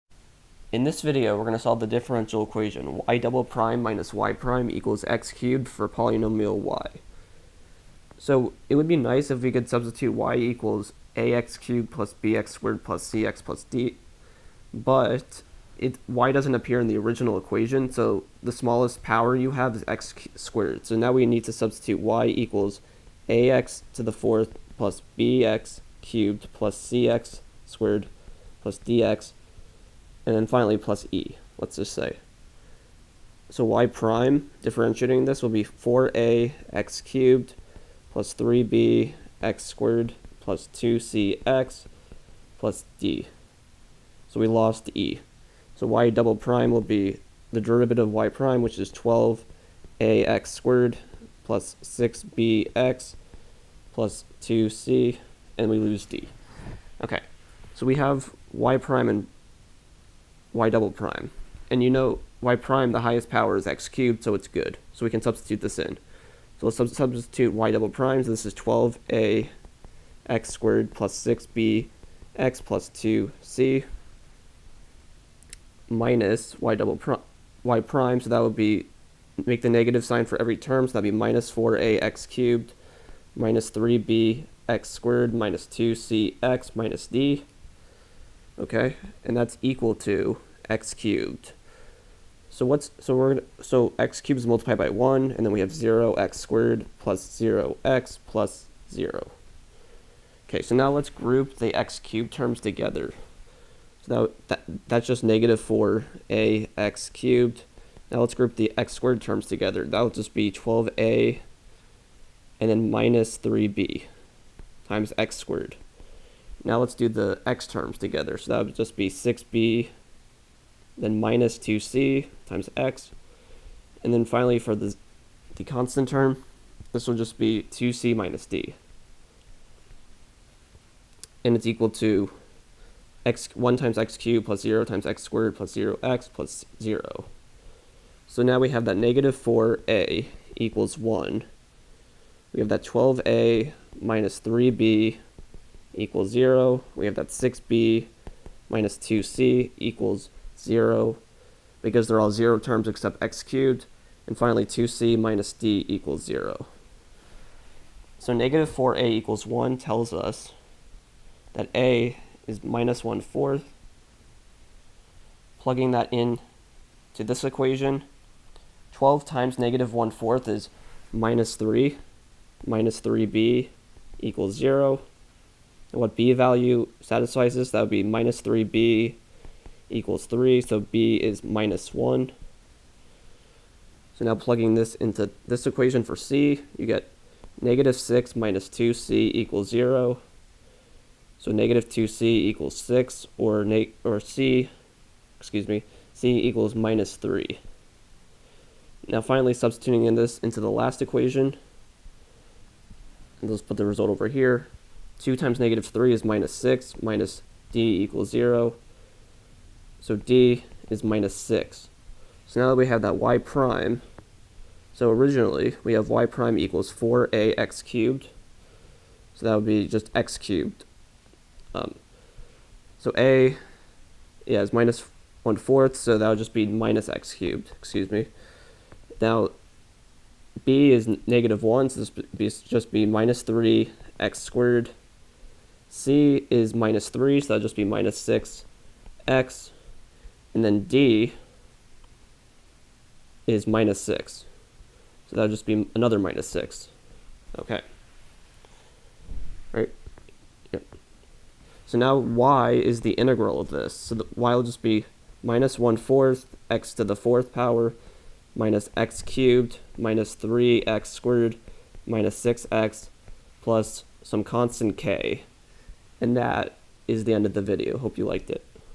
Differential Equations are back!
We solve the differential equation y"-y'=x^3 for polynomial y.